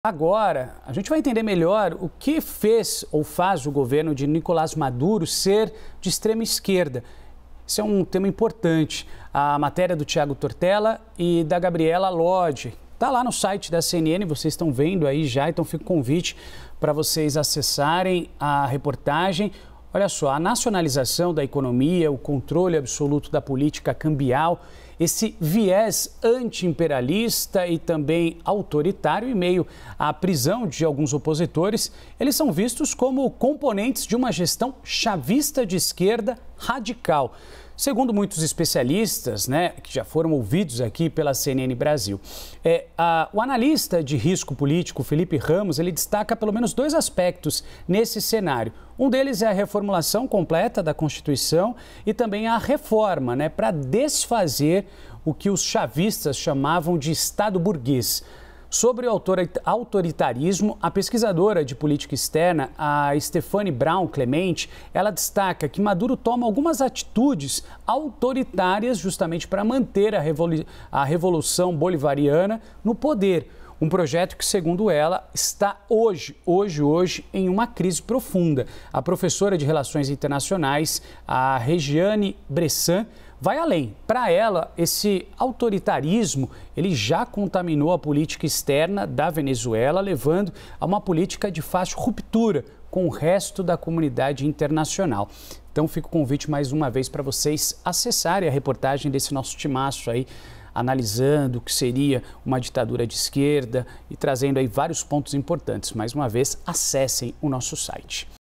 Saiba o que faz governo Maduro ser da extrema-esquerda| AGORA CNN
No poder desde 2013, Nicolás Maduro dá continuidade ao regime de Hugo Chávez, chamado de “socialismo do século XXI”. À CNN, especialistas explicam o que faz o governo de Maduro ser considerado da extrema-esquerda.